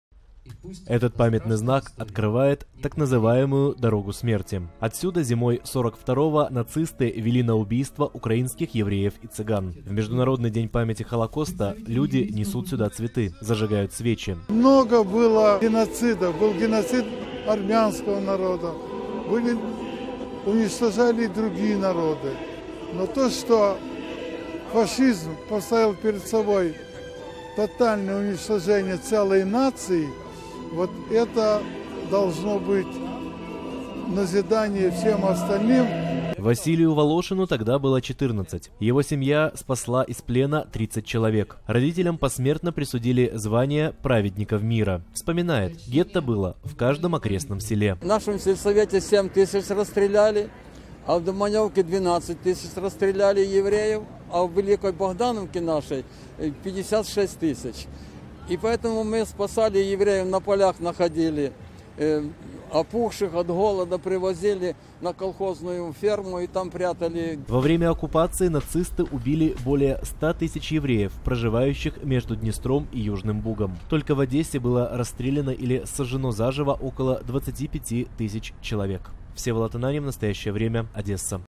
Международный День памяти жертв Холокоста
Сегодня Международный День памяти жертв Холокоста. В Одессе, на единственной в стране аллее праведников мира – так называют людей, которые спасали евреев – собрались свидетели трагедии